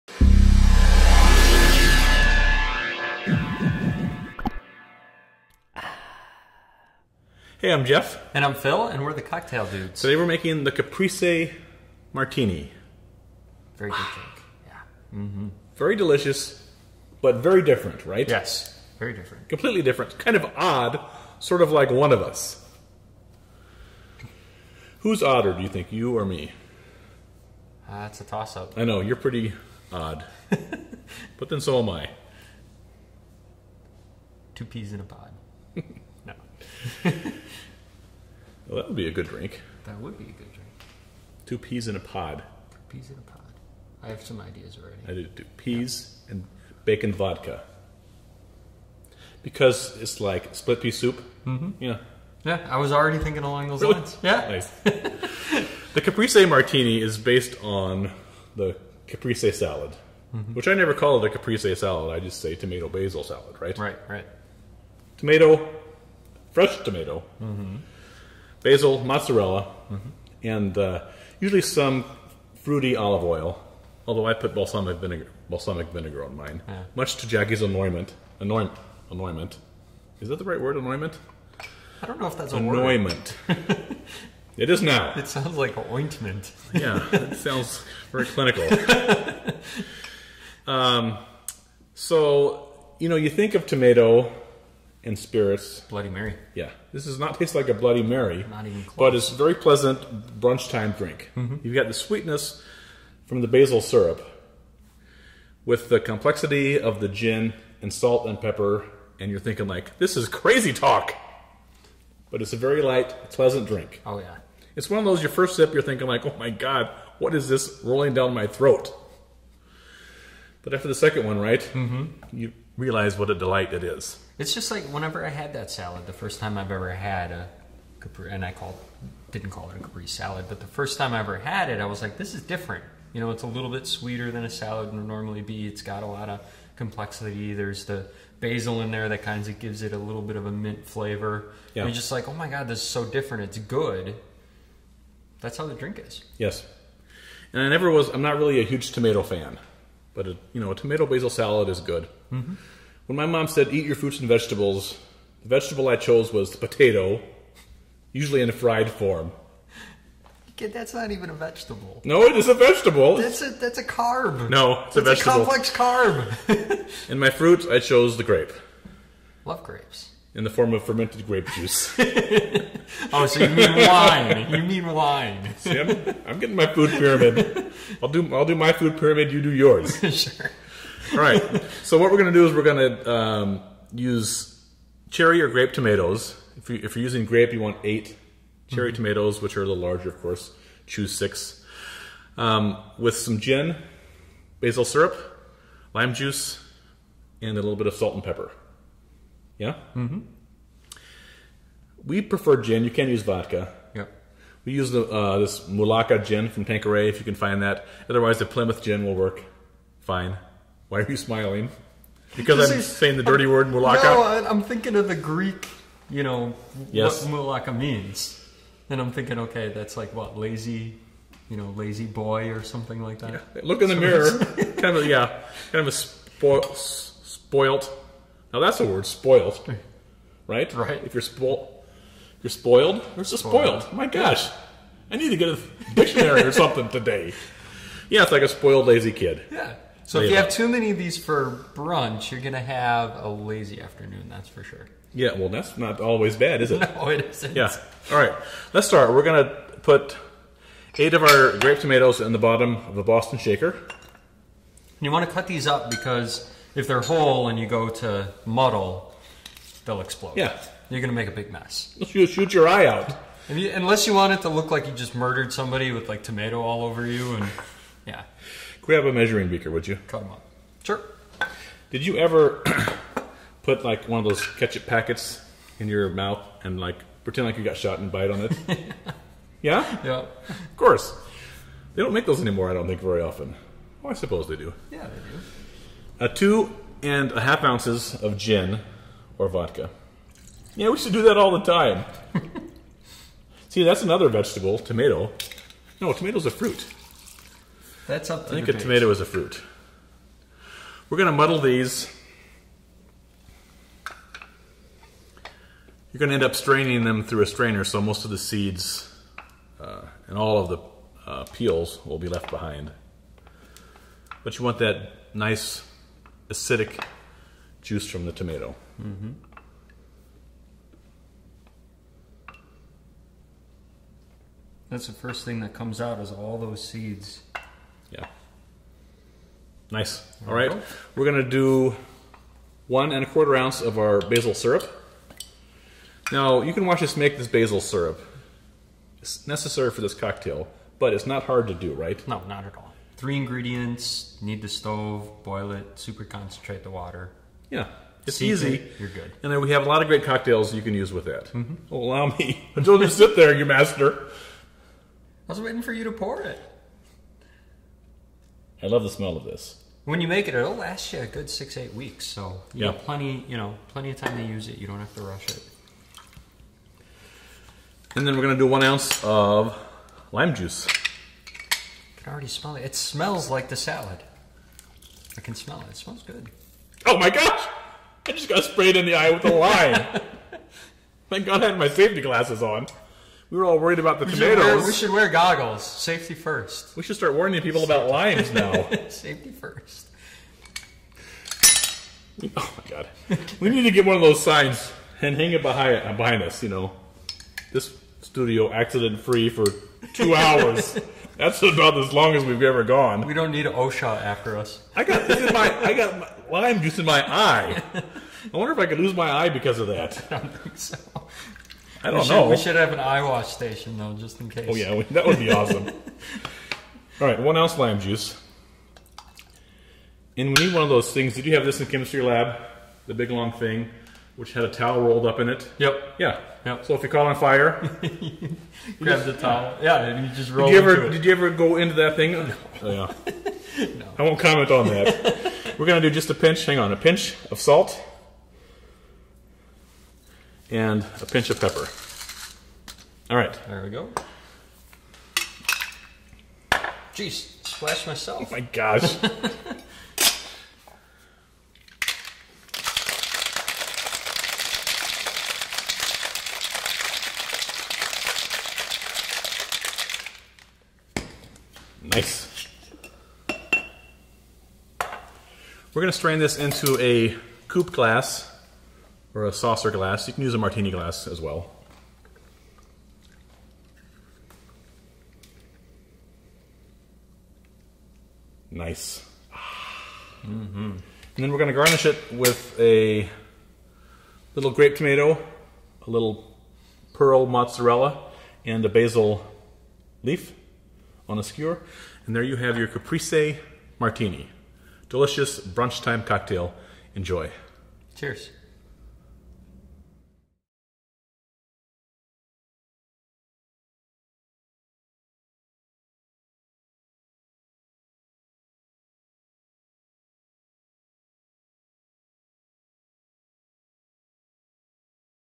The Cocktail Dude's Caprese Martini
Our Caprese Martini is a very unique and delicious cocktail. Aside from pasta, few things go better with tomatoes than basil. What we have here is a caprese salad in a cocktail glass! Though we literally mess up the tomatoes by muddling them, we completely finish them off by drowning them in gin. This is an excellent brunch cocktail. More delicate than your common tomato-based Bloody Mary, enjoy this cocktail along with the others which utilize our basil syrup. Cheers!